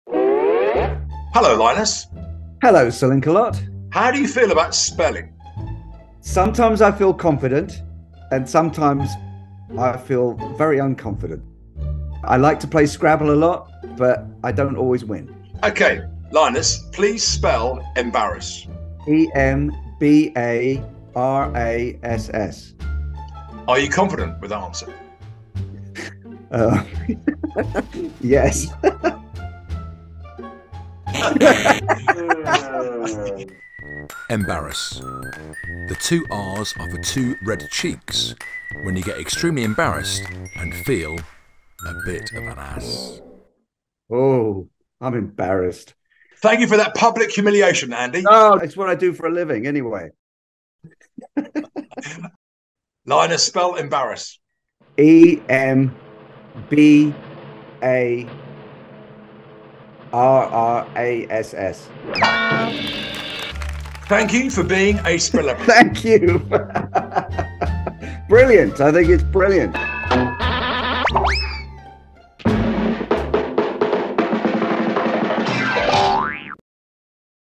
"I'm a spellebrity!" // Linus Roache
Our next Spellebrity is Golden Globe-nominated actor Linus Roache, son of Corrie legend William, who, of course, had his security blanket as he was a bit 'nerve' about his spelling. At least he wasn't sucking his thumb.